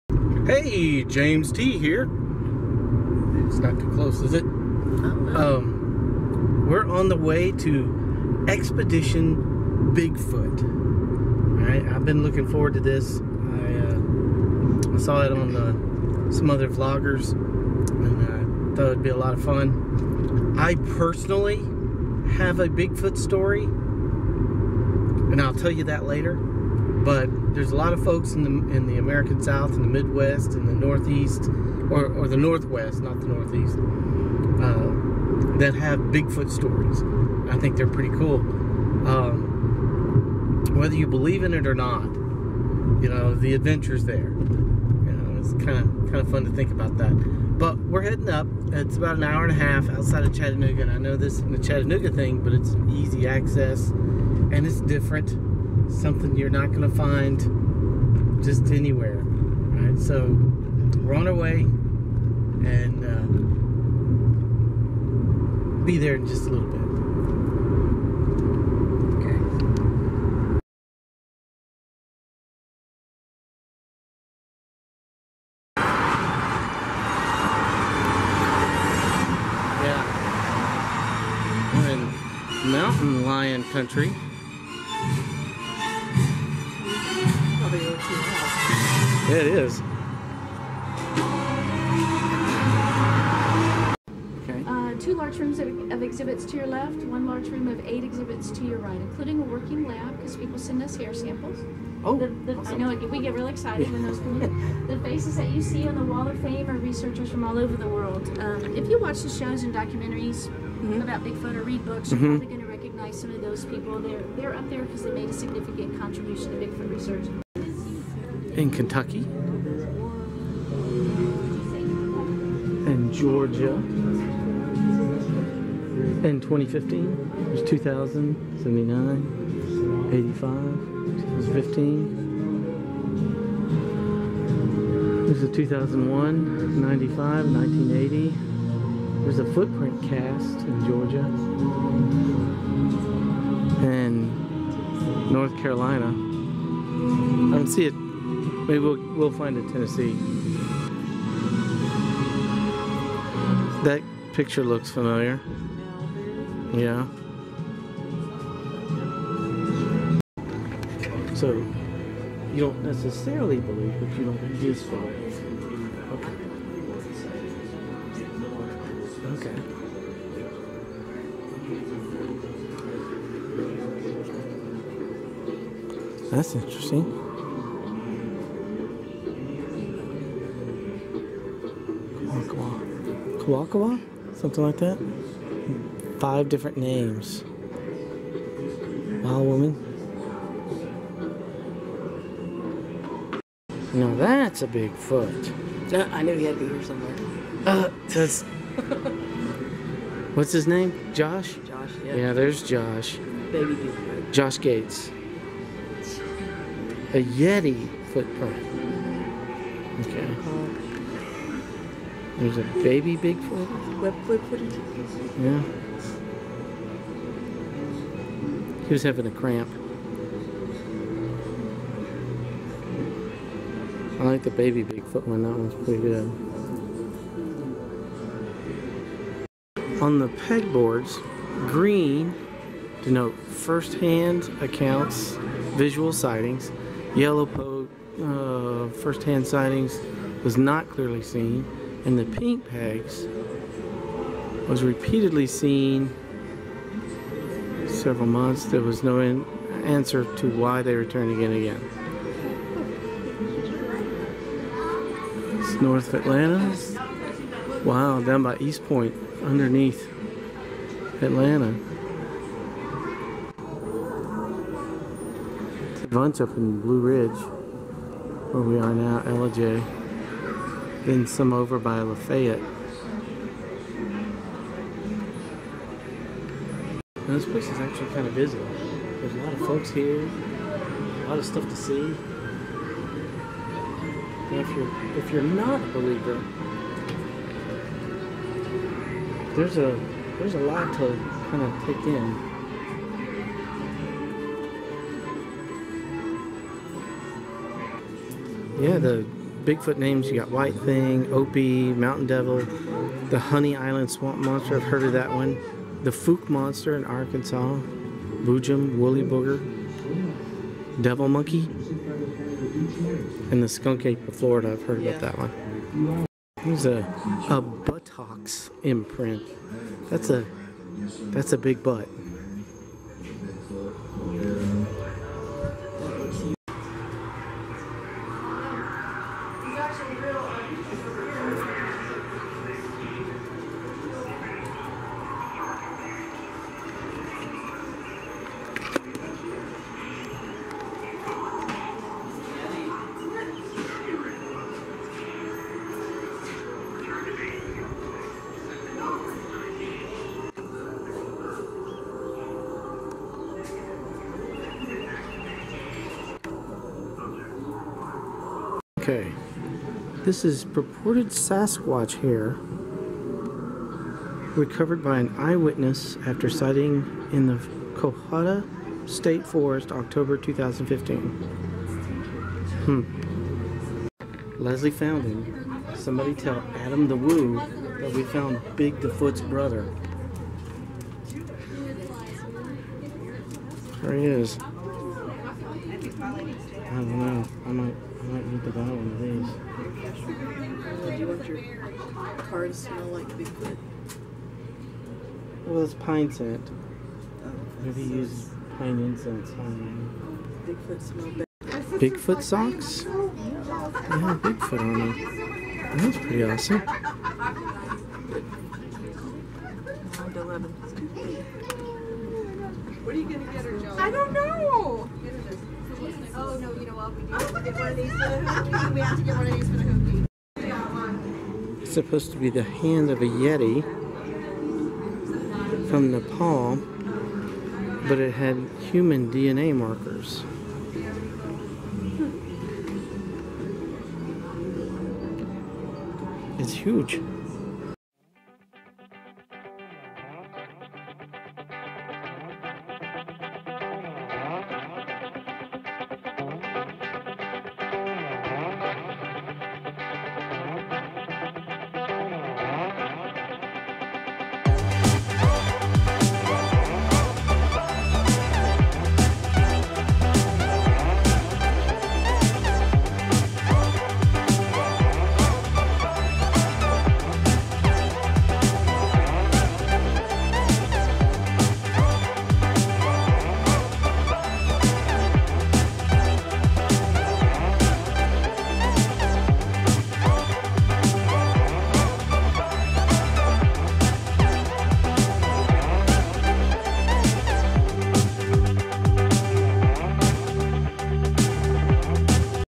Expedition Bigfoot
We're on the road to Cherry Log Georgia. Let's check out the Expedition Bigfoot facility there, you never know we might just find something we least expect.